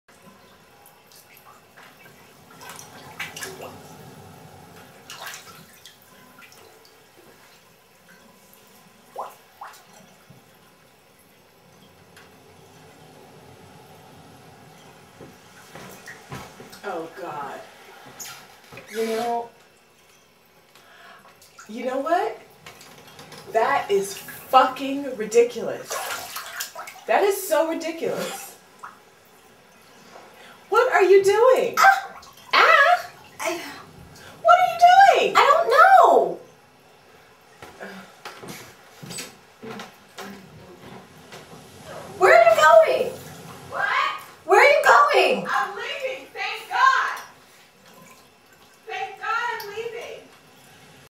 Taking A Bath In A Cocktail Dress- Do You Think I'm Kidding
Gina's housemateTobe comes in to the bathroom to find Gina in the bathtub with her clothes on. Come visit me.